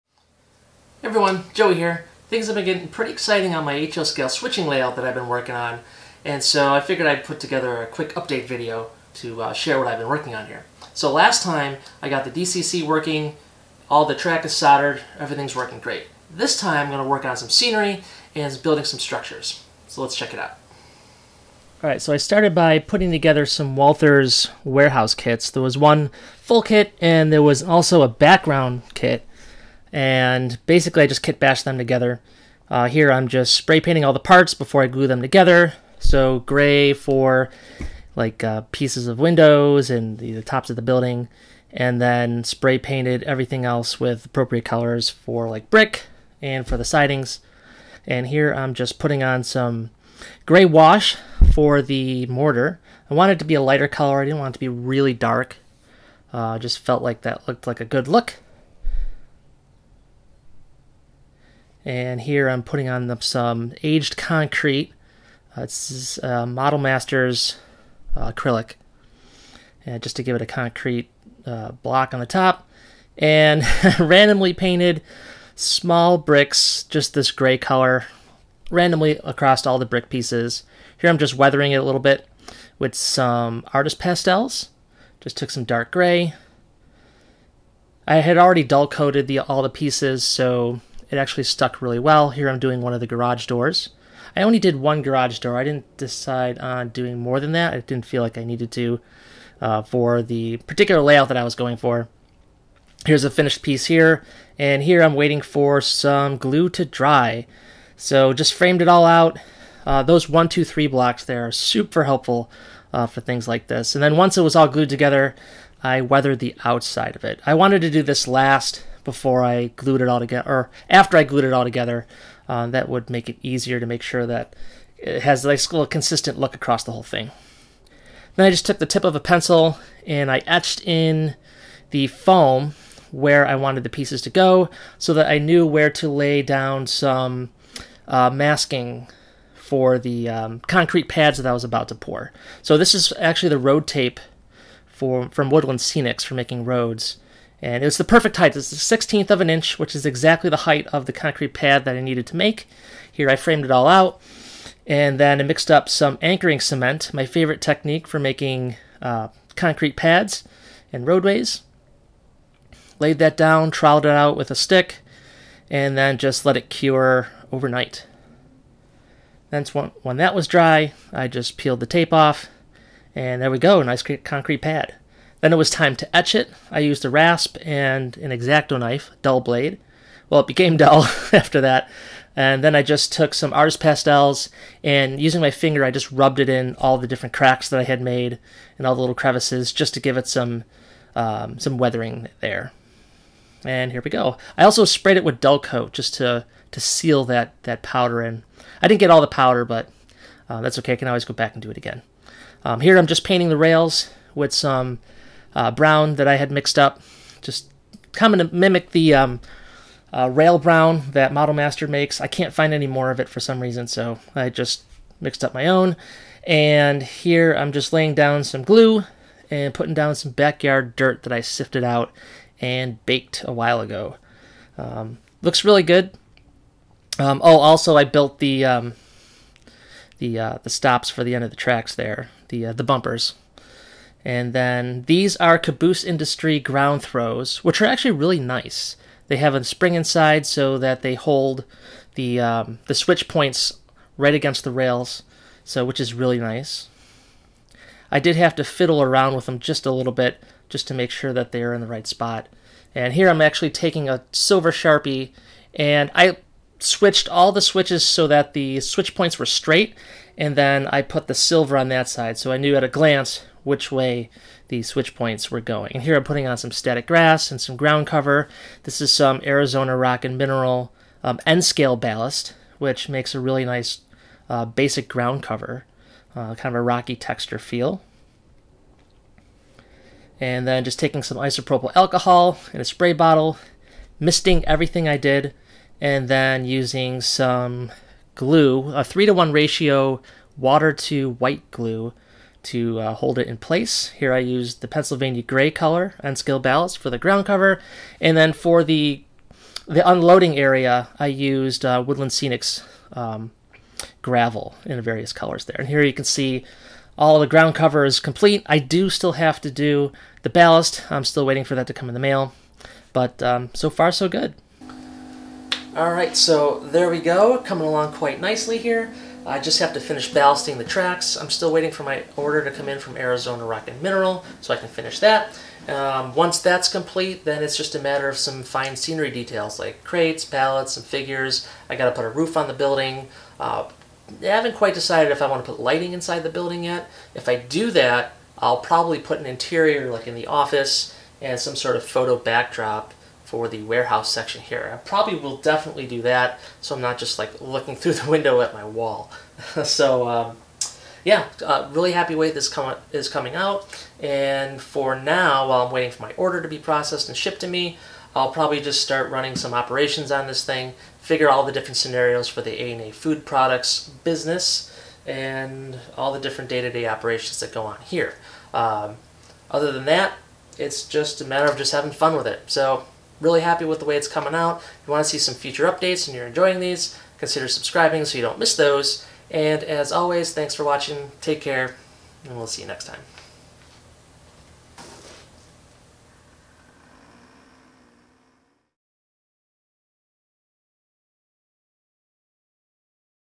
HO Scale Switching Layout Build - A&A Foods Products - Part 3
Exciting new scenery on the HO Scale switching layout! I finally got around to creating my first kitbashed structure, and threw down some ground cover and other scenery. Enjoy!